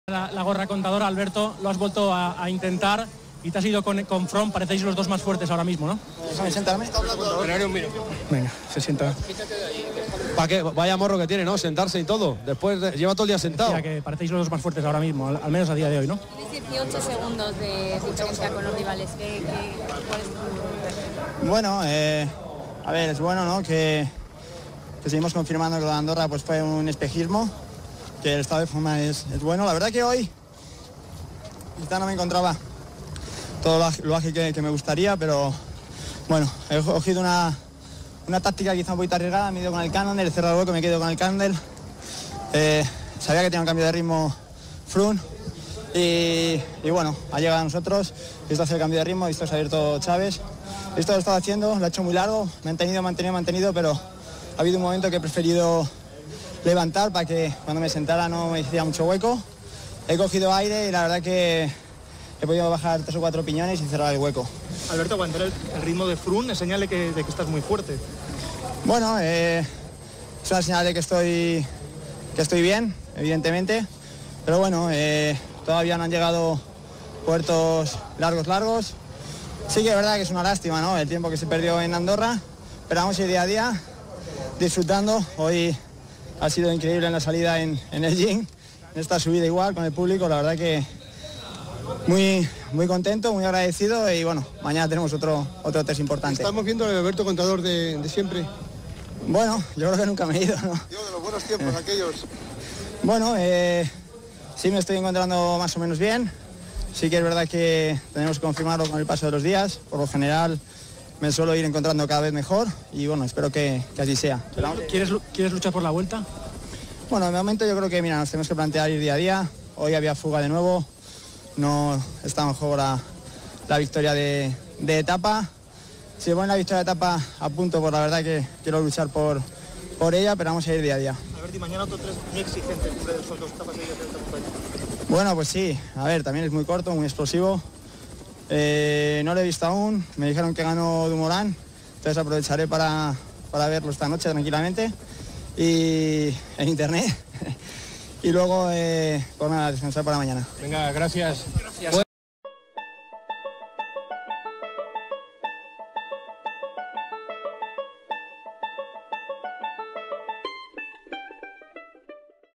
Alberto Contador | FUE UNA PENA LO DE ANDORRA | Etapa 8 | Xorret Cadí | Vuelta a España 2017
Alberto Contador en declaraciones tras finalizar la 8a etapa de esta Vuelta a España 2017.